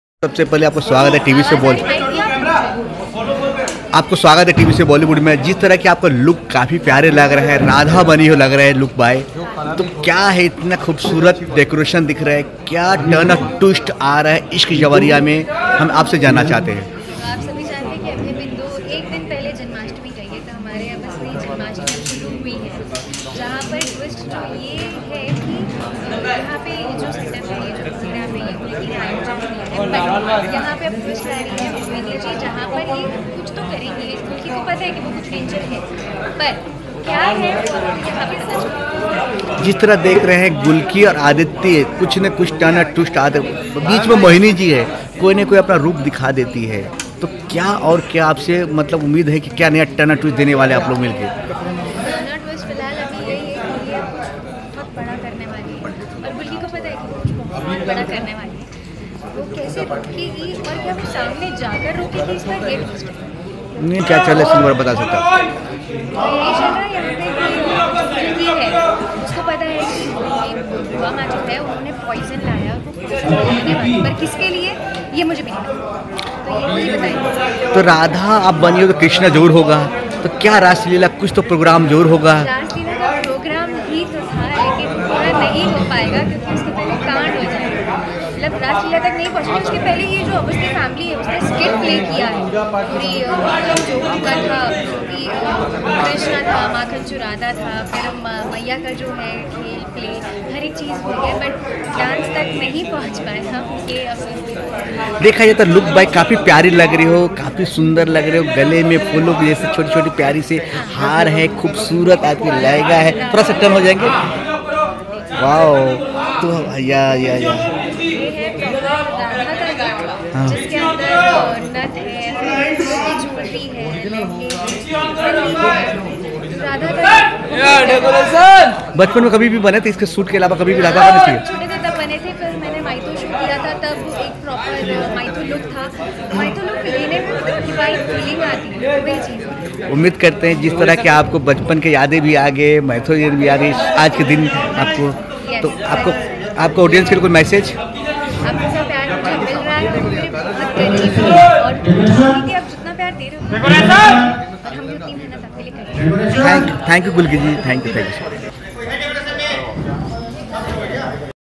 Ishq Jabariya Onset Gulki Aditya Mohini ke love❤️story Tention Husband ke किया सोच रही हैonlocation
Neo Tv channel PE Dekho Full Episode family ke sath Enjoy Karo Ishq Jabariya Onset

Exclusive interview video Tv se Bollywood Tak